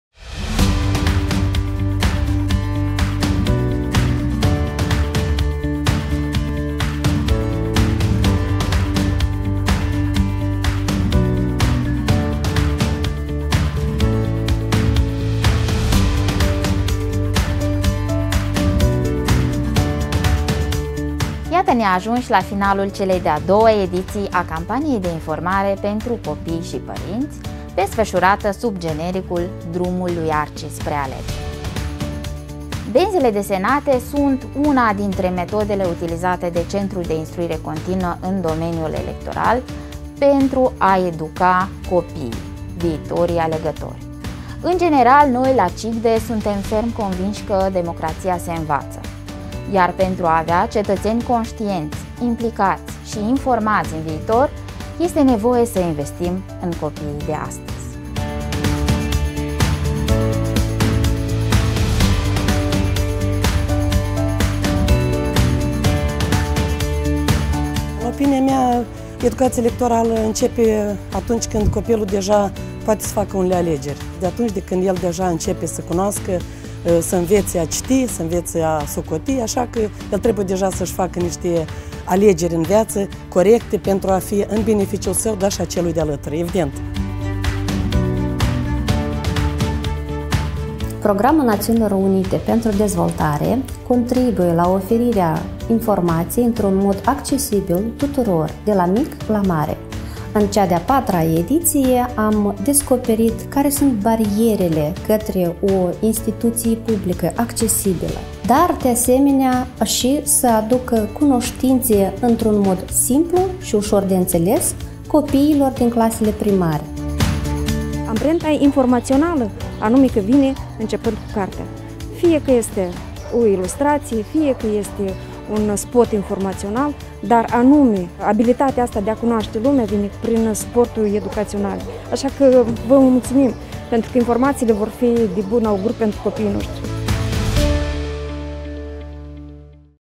About 15,800 children received the electoral comics "Arci promotes elections without barriers"
The hedgehog Arci’s bus and the Centre for Continuous Electoral Training
volunteers reached 130 schools in 113 communities from 11 districts of Moldova, during February-March 2021. About 15,800 primary school children
received the fourth edition of the electoral comics, entitled "Arci promotes elections without barriers".

The schools received the latest two editions of the electoral comics with the hedgehog Arci, the brochure "Democracy can be learned", board games, the mascot Arci and even the Constitution of the Republic of Moldova.

The mobile campaign "Arci's Journey to Elections" was developed within the Project "Enhancing democracy in Moldova through inclusive and transparent elections", implemented by UNDP Moldova, with the financial support of the American people, provided through the United States Agency for International Development (USAID).